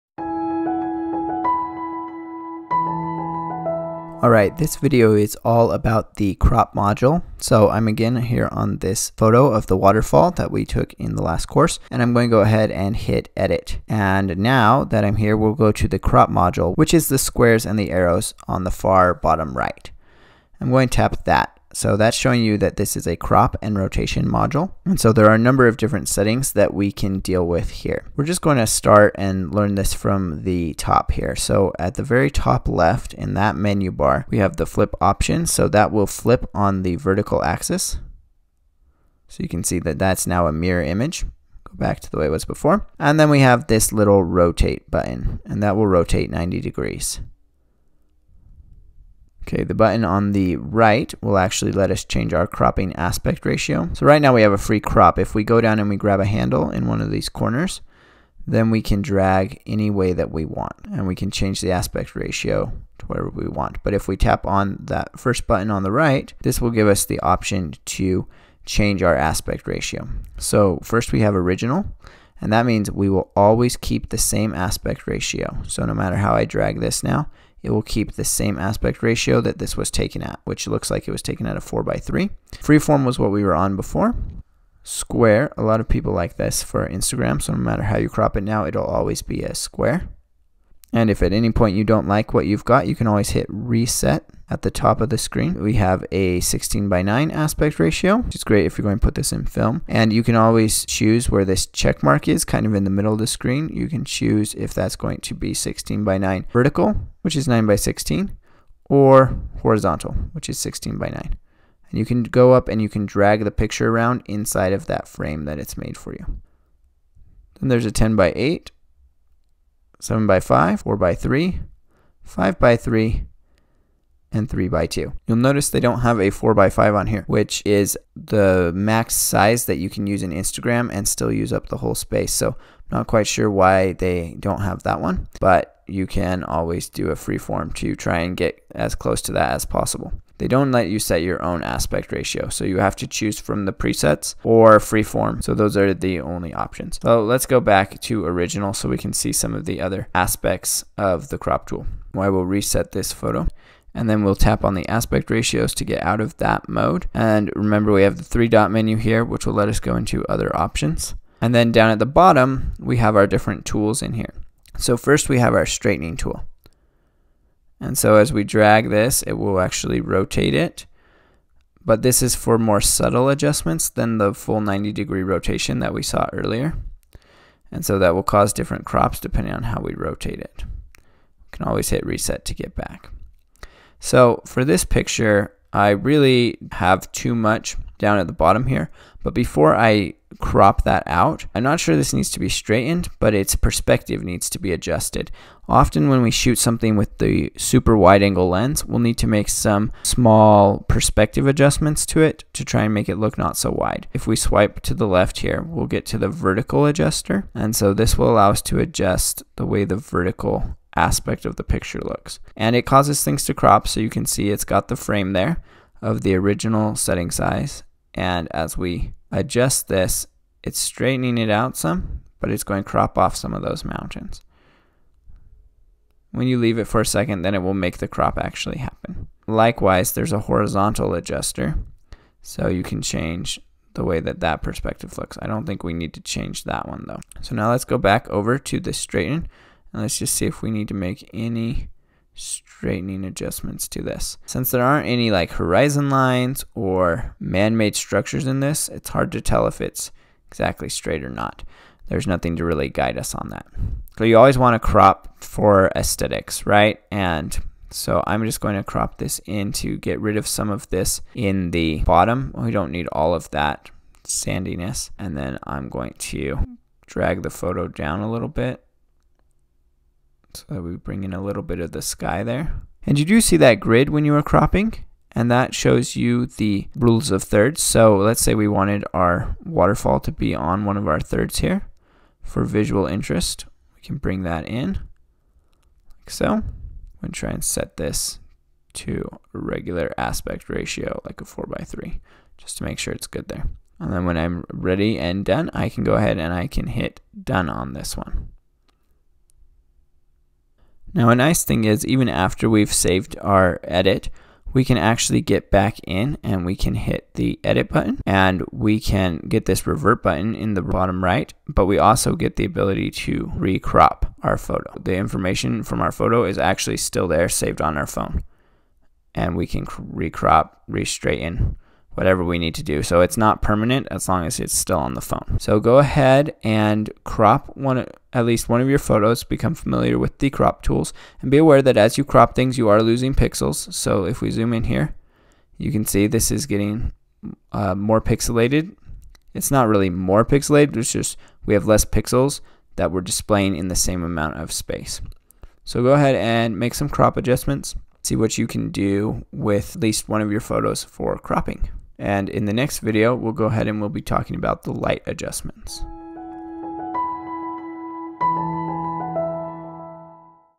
Cropping Photos on iPhone 11 and 11 Pro
In this video we look at all the cropping options in the photos app on iPhone. This is a powerful way to reframe your shot.
This is part of a whole course on iPhone Photo Editing that you can find